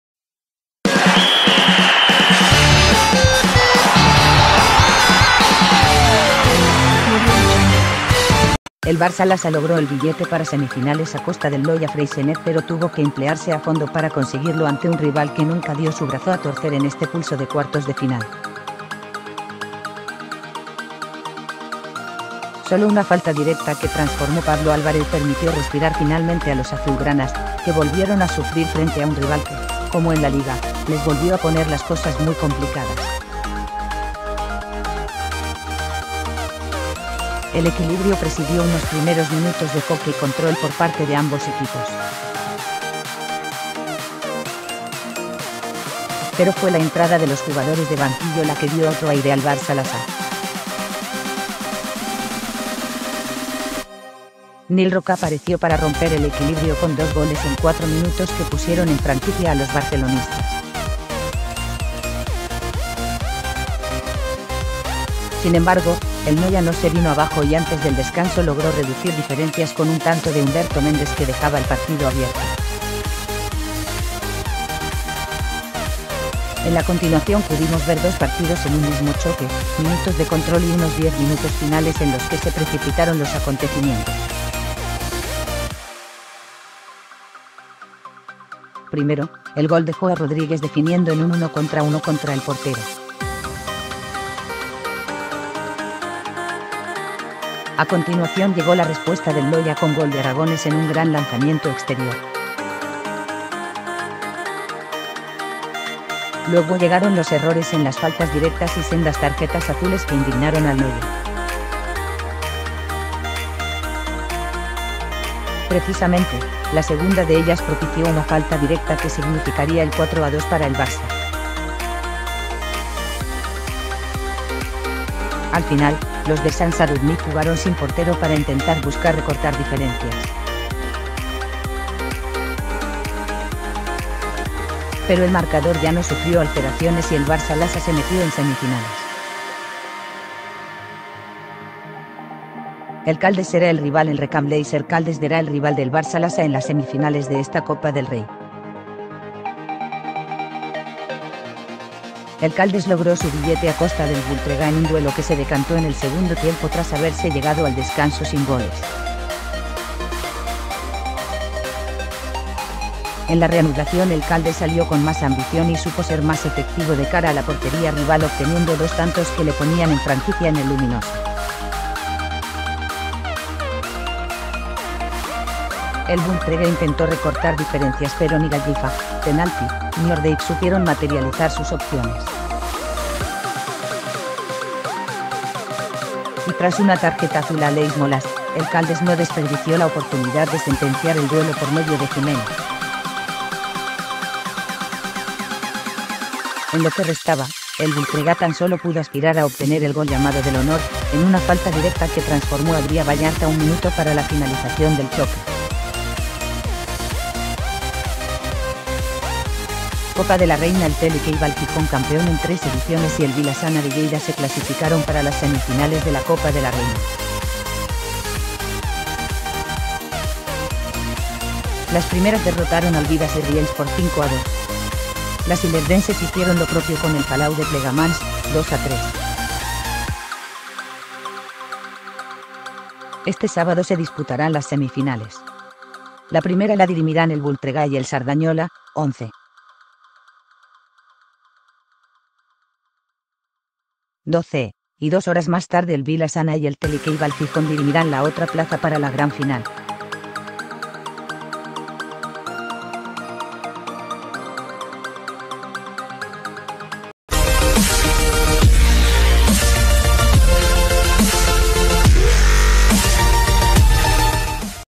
El Barça Lassa tuvo que emplearse a fondo para pasar a ＇semis＇
El Barça Lassa logró el billete para semifinales a costa del Noia Freixenet pero tuvo que emplearse a fondo para conseguirlo ante un rival que nunca dio su brazó a torcer en este pulso de cuartos de final.   Sólo una falta directa que transformó Pablo Álvarez permitió respirar finalmente a los azulgranas, que volvieron a sufrir frente a un rival que, como en la Liga, les volvió a poner las cosas muy complicadas.   El equilibrio presidió unos primeros minutos de hockey- control por parte de ambos equipos. Pero fue la entrada de los jugadores de banquillo la que dio otro aire al Barça Lassa. Nil Roca apareció para romper el equilibrio con dos goles en cuatro minutos que pusieron en franquicia a los barcelonistas.Sin embargo, el Noia no se vino abajo y antes del descanso logró reducir diferencias con un tanto de Humberto Mendes que dejaba el partido abierto.  En la continuación pudimos ver dos partidos en un mismo choque: minutos de control y unos diez minutos finales en los que se precipitaron los acontecimientos.  Primero, el gol de Joao Rodrigues definiendo en un uno contra uno contra el portero. A continuación llegó la respuesta del Noia con gol de Aragonès en un gran lanzamiento exterior. Luego llegaron los errores en las faltas directas y sendas tarjetas azules que indignaron al Noia. Precisamente, la segunda de ellas propició una falta directa que significaría el 4-2 para el Barça.  Al final,  los de Sant Sadurní jugaron sin portero para intentar buscar recortar diferencias. Pero el marcador ya no sufrió alteraciones y el Barça Lassa se metió en semifinales..  El Caldes será el rival  El Recam Làser Caldes derá el rival del Barça Lassa en las semifinales de esta Copa del Rey. El Caldes logró su billete a costa del Voltregà en un duelo que se decantó en el segundo tiempo tras haberse llegado al descanso sin goles.  En la reanudación el Caldes salió con más ambición y supo ser más efectivo de cara a la portería rival obteniendo dos tantos que le ponían en franquicia en el luminoso.  El Voltregà intentó recortar diferenciaspero ni Gallifa (penalty) ni Ordeig supieron materializar sus opciones. Y tras una tarjeta azul a Aleix Molas, el Caldes no desperdició la oportunidad de sentenciar el duelo por medio de Giménez.  En lo que restaba, el Voltregà tan sólo pudo aspirar a obtener el gol llamado del honor, en una falta directa que transformó Adrià Ballart a un minuto para la finalización del choque.  Copa de la Reina  El Telecable Gijón -campeón en tres ediciones- y el Vila-Sana de Lleida se clasificaron para las semifinales de la Copa de la Reina. Las primeras derrotaron al Bigues i Riells por 5-2.  Las ilerdenses hicieron lo propio con el palau de Plegamans (2-3). Este sábado se disputarán las semifinales. La primera la dirimirán el Voltregà y el Cerdanyola (11.00 h.) y dos horas más tarde el Vila-Sana y el Telecable Gijón dirimirán la otra plaza para la gran final.